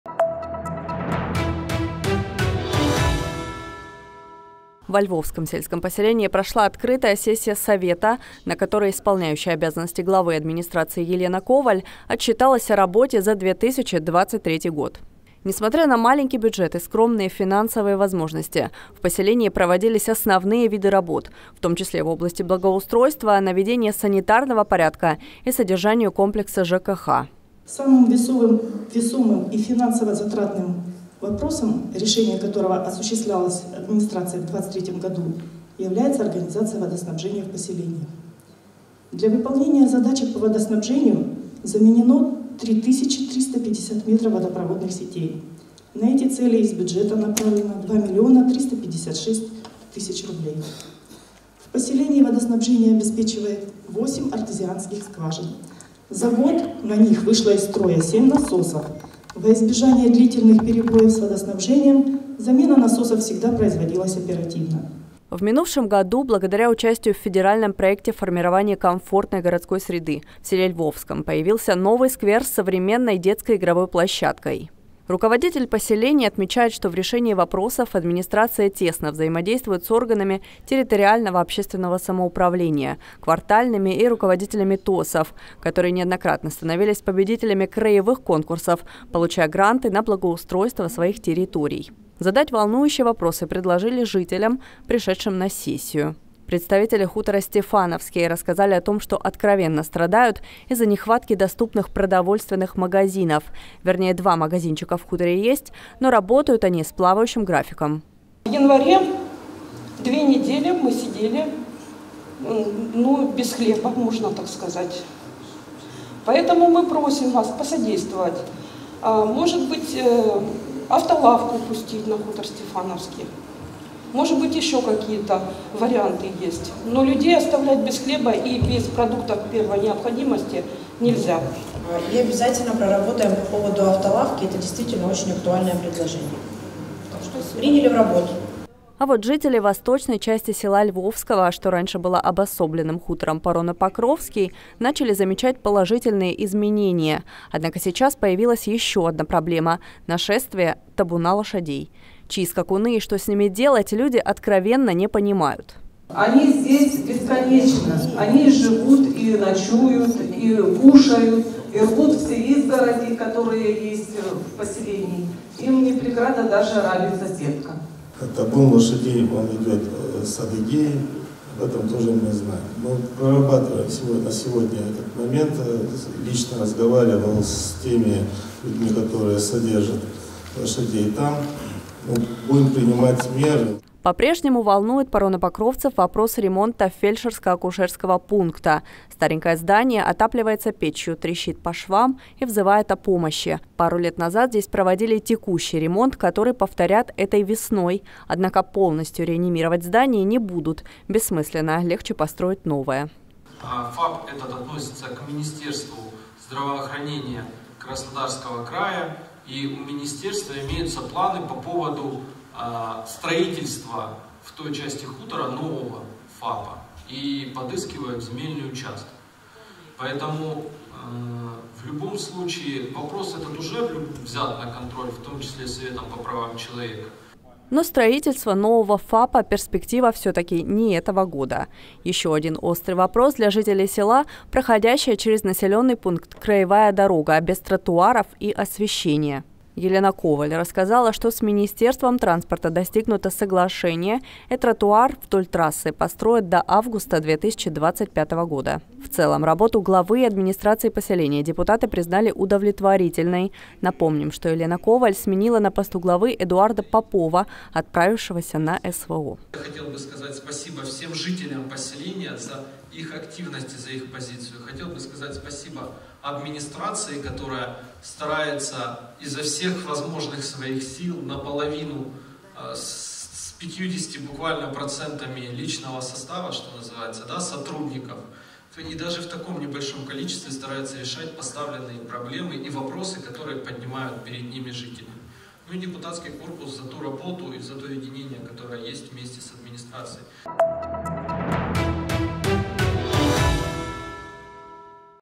Во Львовском прошла открытая сессия Совета поселения
Исполняющая обязанности главы администрации Елена Коваль отчиталась о работе за 2023 год. Несмотря на маленький бюджет и скромные финансовые возможности в поселении проводились основные виды работ, в том числе в области благоустройства, наведения санитарного порядка и содержания комплекса ЖКХ.